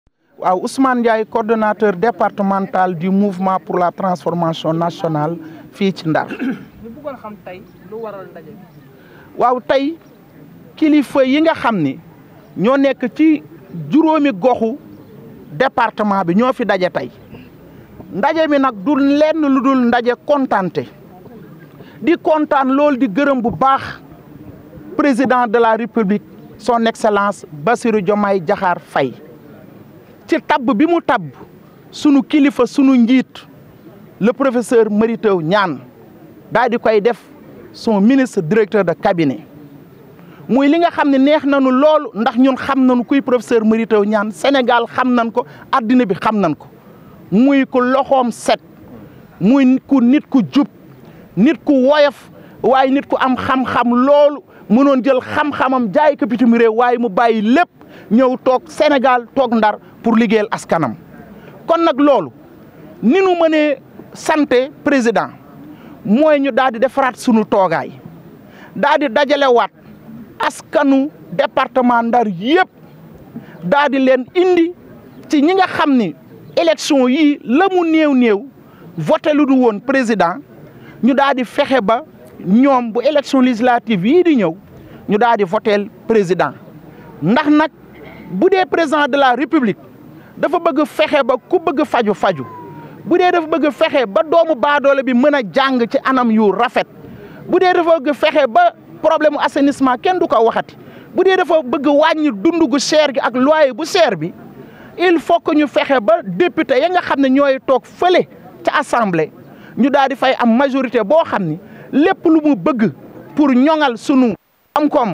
Le mouvement MTN compte accompagner le gouvernement du 1er ministre Ousmane Sonko
Les responsables du Mouvement de Transformation National (MTN) du professeur Mary Teuw Niane se sont retrouvés en assemblée générale afin de massifier le mouvement pour les prochaines joutes électorales. Ils ont profité de l'occasion pour remercier le Président Bassirou Diomaye Diakhare Faye pour son choix porté sur leur leader, tout en promettant d'accompagner le gouvernement du premier ministre Ousmane Sonko dans son programme de mener à bon port notre pays le Sénégal.